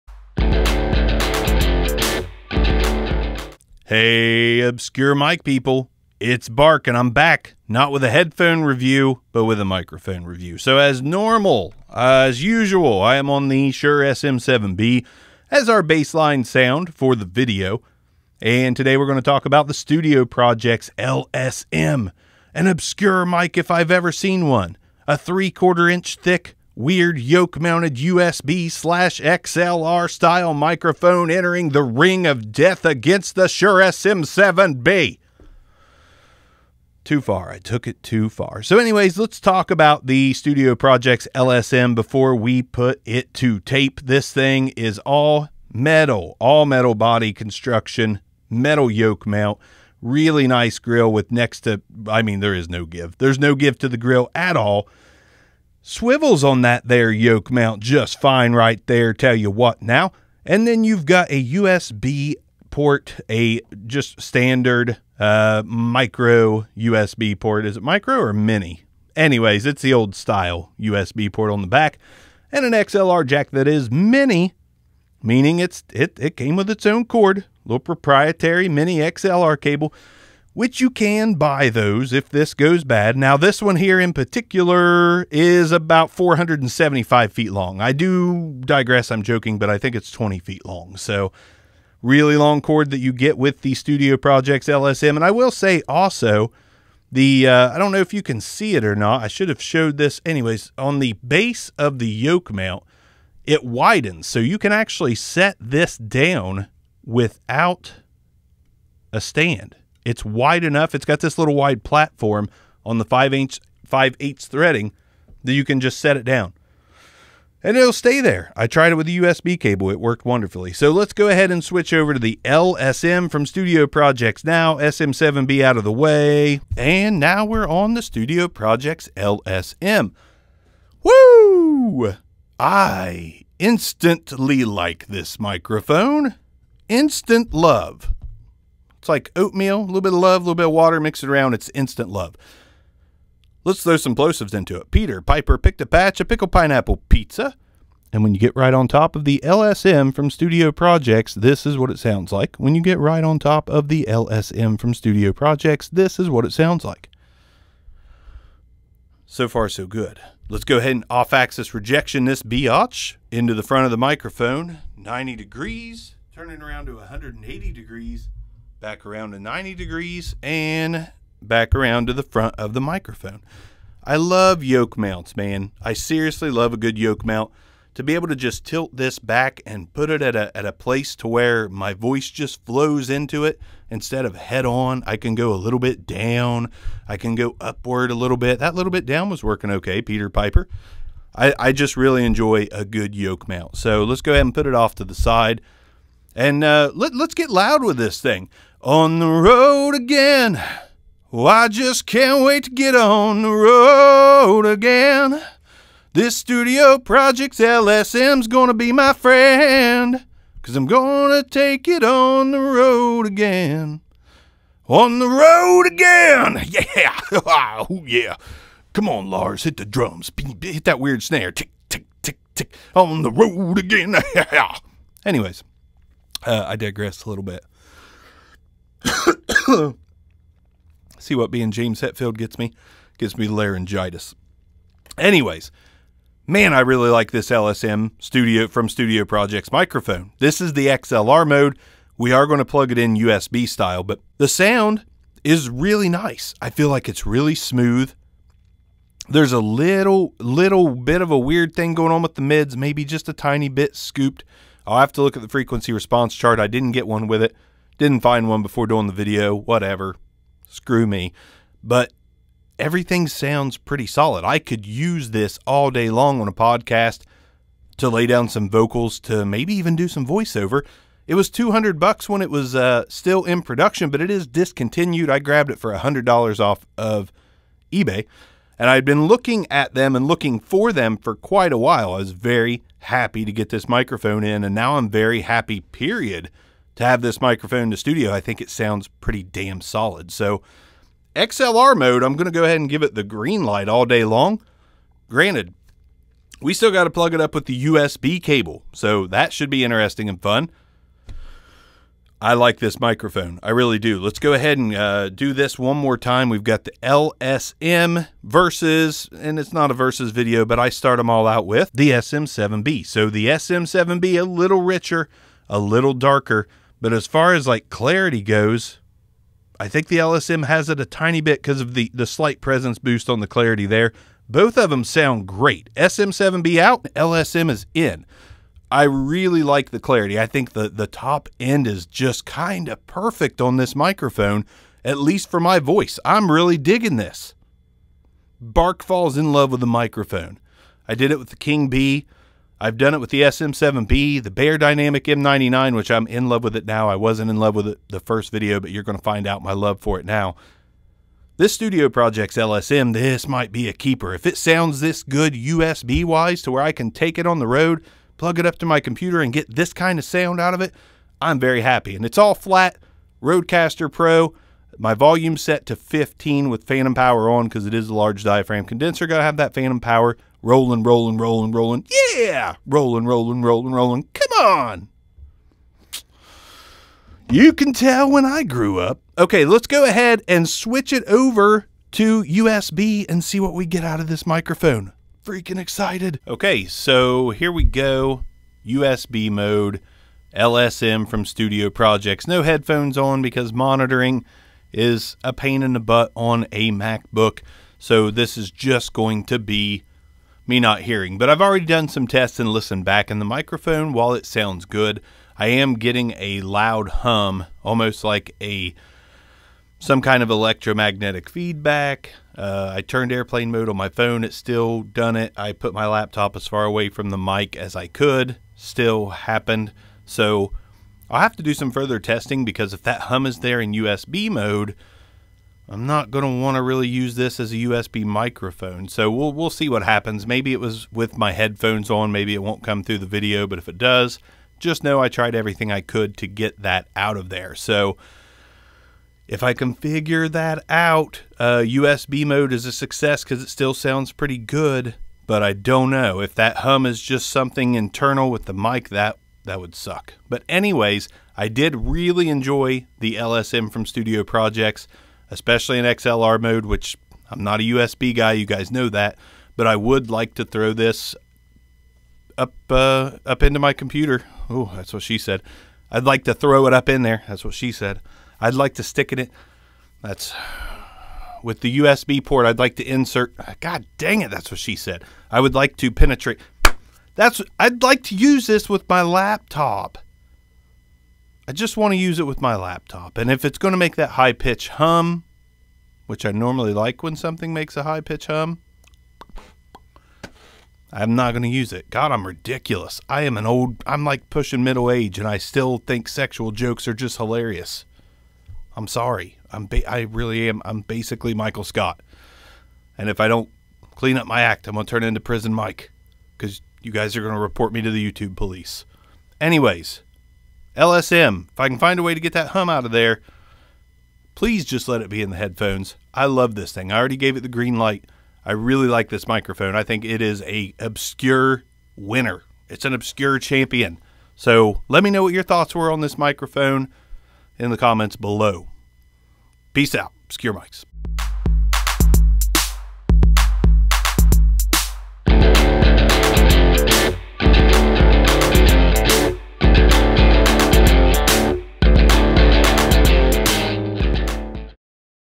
Studio Projects LSM | XLR & USB Condenser Microphone | Test & Review with Shure SM7B Opening
Studio Projects created a mic like no other. at least not that I've seen. This thing is cool. XLR AND USB... versatile.

BUY LSM - New Studio Projects LSM Pink Microphone Little Square Microphone Diaphragm Condenser Mic